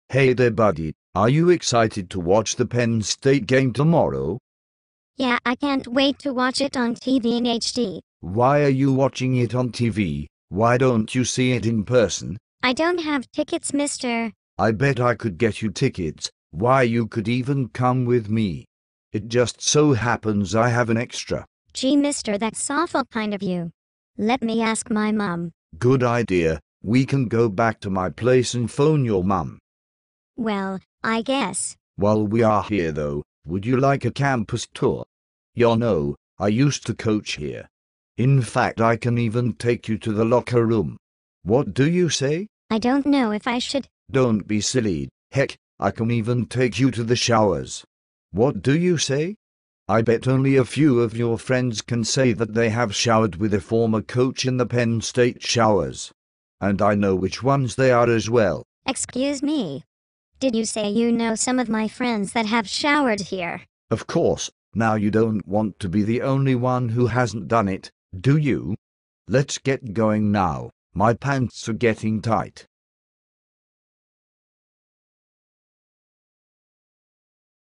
Shocking footage of Jerry Sandusky with young boy at Penn State! Leaked by media earlier today!!
Tim Curley and Gary Schultz covered up numerous sexual encounters that Jerry Sandusky had with boys during his time at PSU. However this shocking footage leaked to the media earlier today.